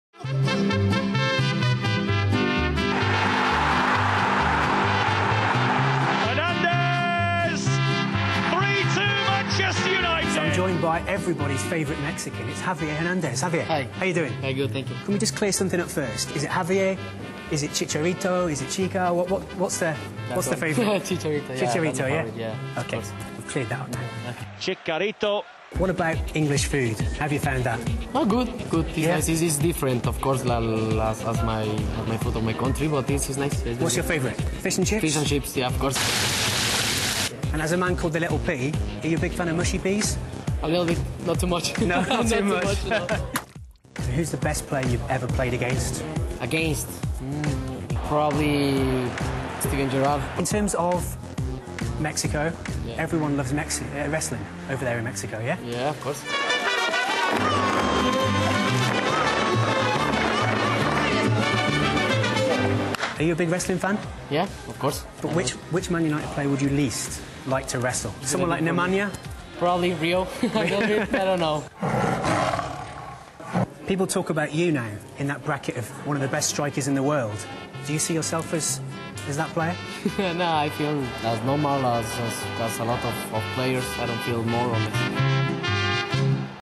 Paz meets... Javier Hernandez
Chicharito? Javier? Little Pea? Paz gets to the bottom of it with Mexico's finest